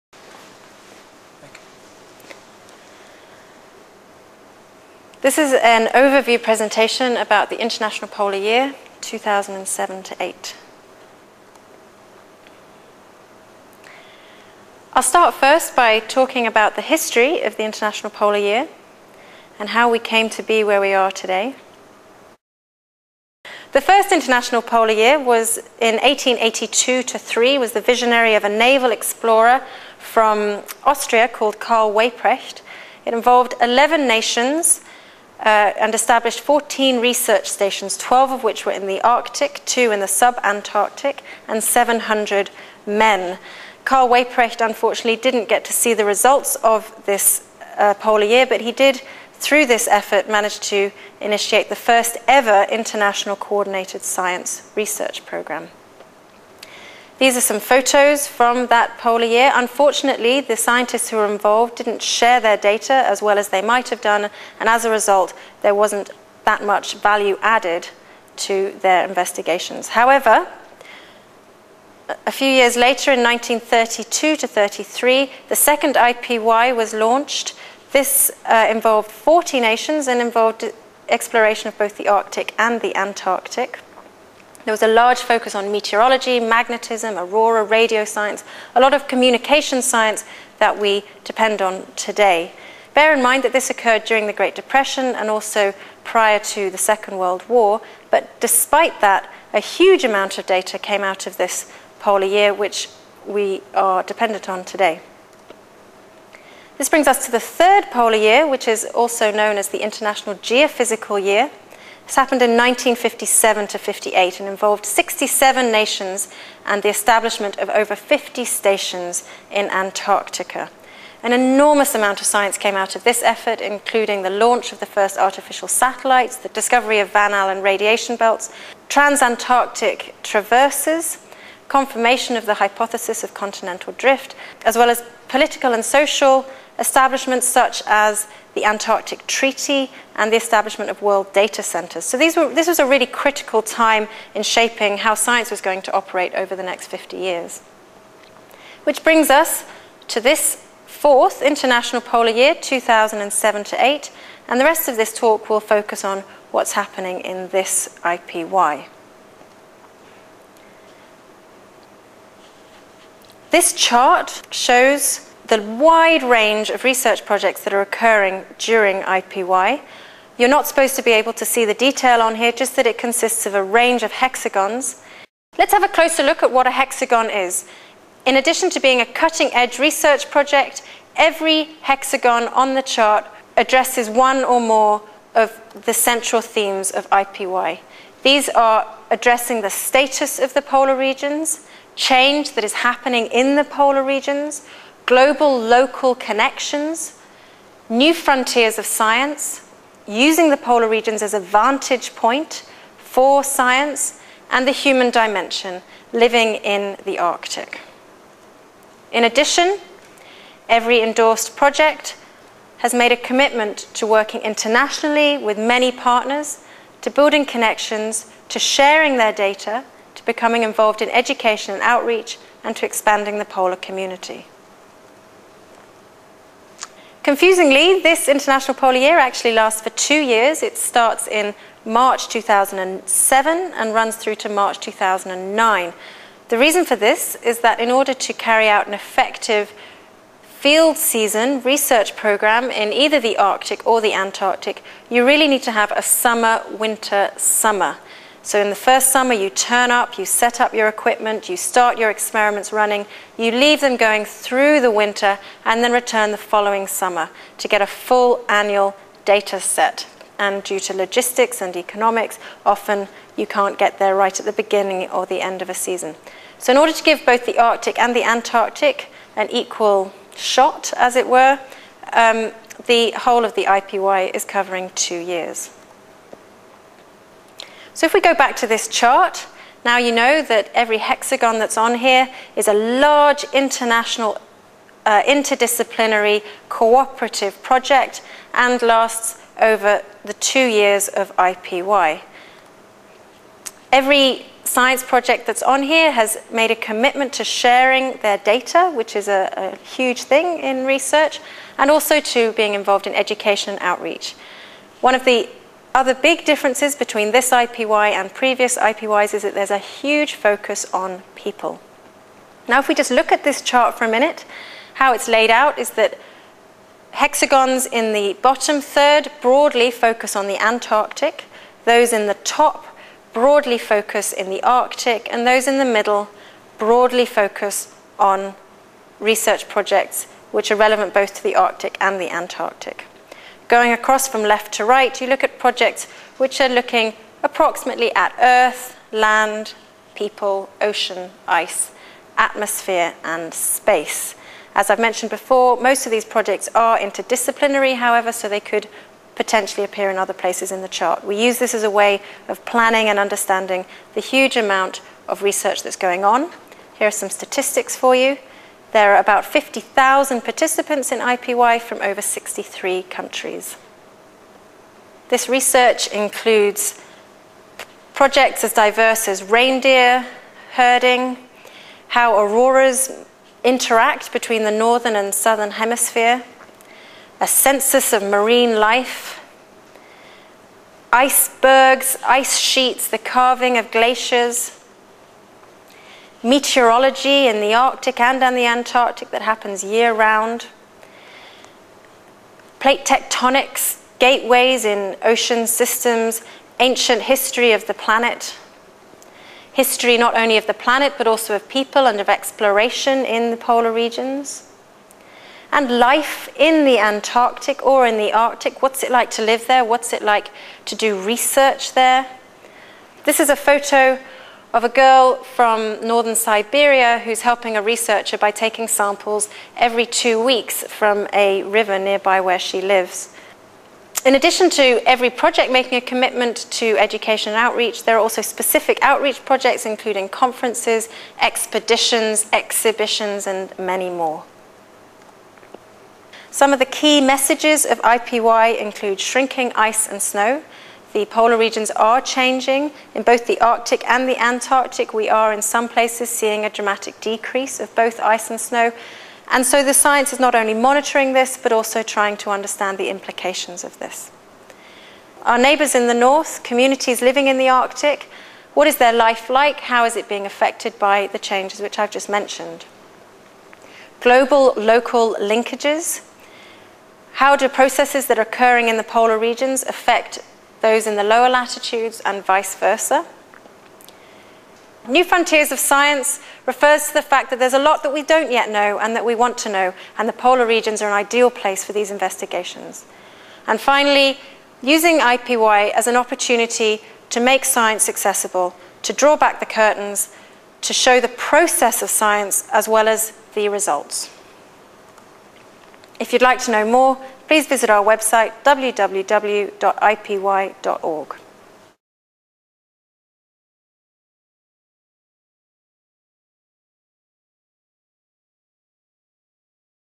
International Polar Year - Overview
Dr Rhian Salmon, IPY Education and Outreach Coordinator, narrates this presentation overview of The International Polar Year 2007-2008